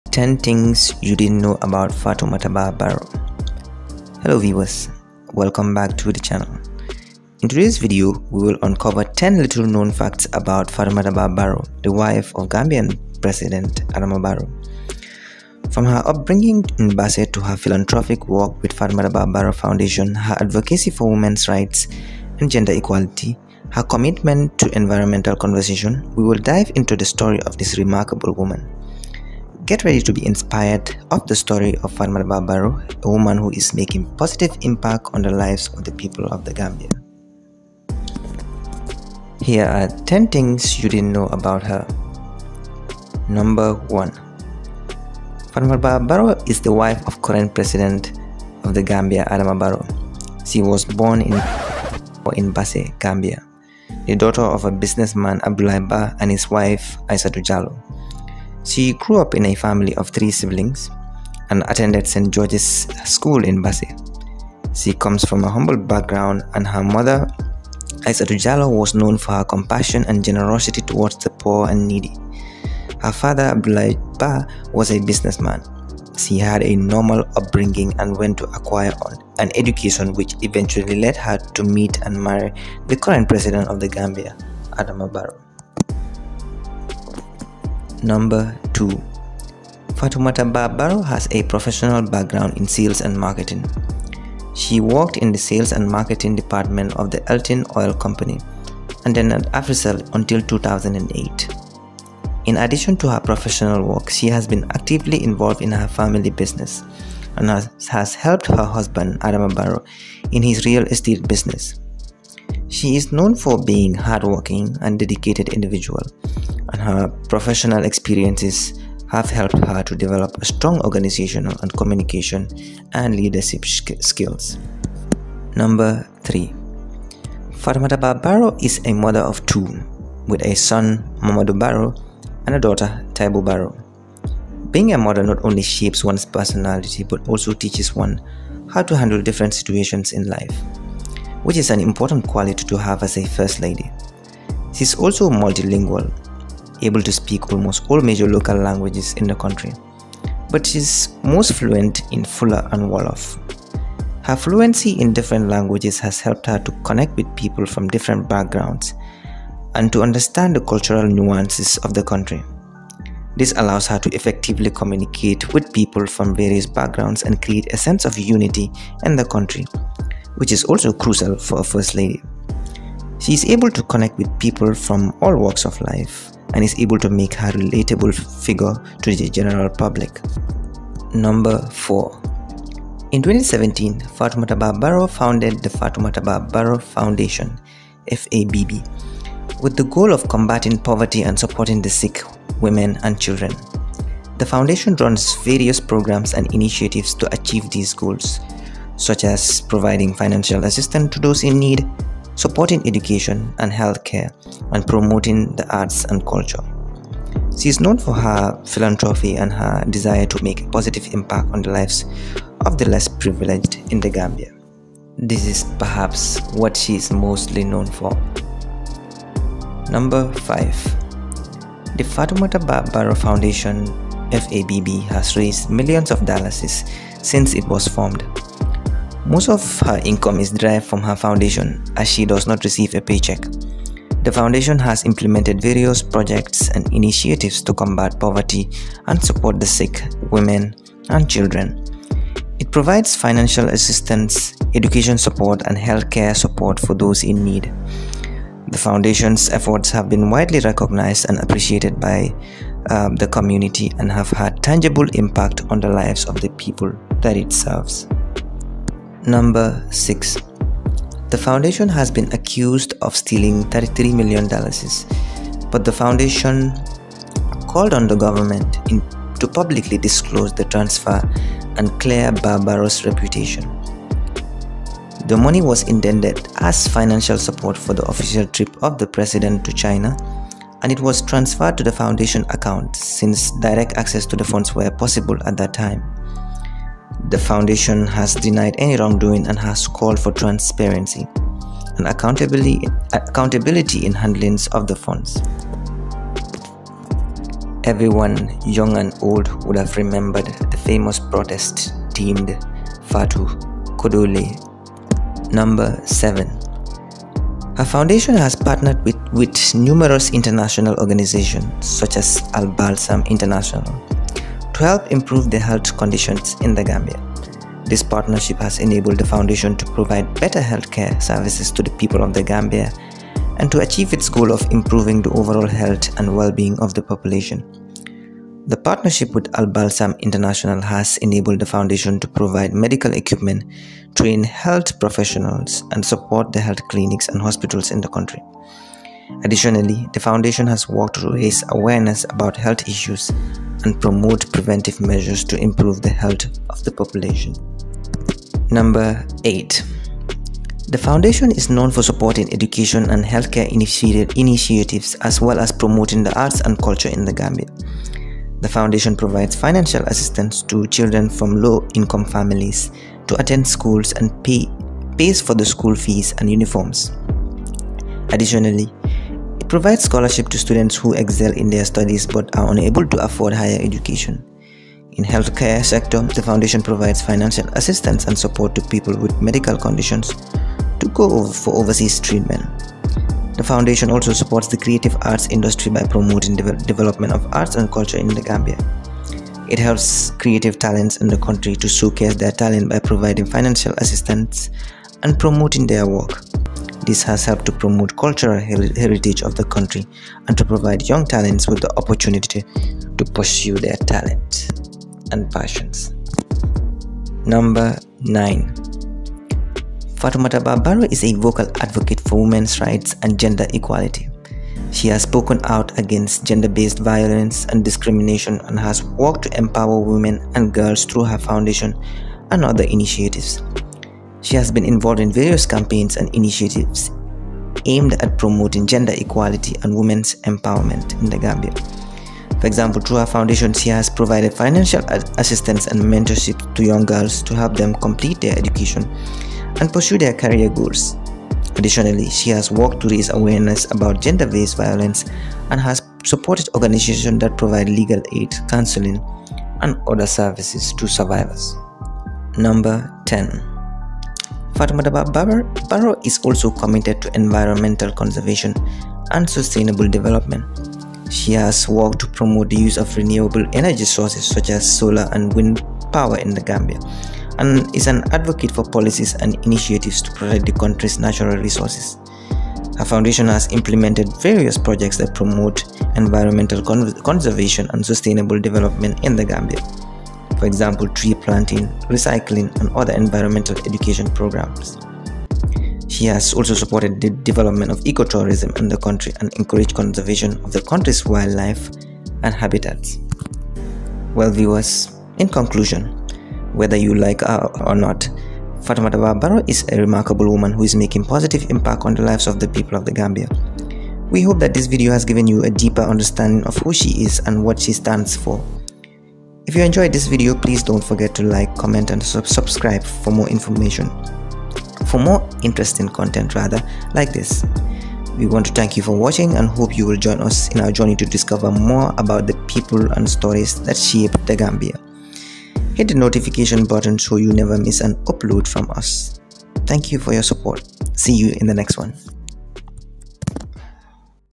Gambia Kachaa: Hidden Facts About First Lady Of The Gambia Fatoumatta Bah Barrow
Gambia Kachaa: The Hidden Facts About First Lady Of The Gambia Fatoumatta Bah Barrow - This video we take a detail look at the life of Fatoumatta Bah Barrow the current First Lady of The Gambia and wife of President Adama Barrow .

Every Gift Ensures Continuity Of Our Work!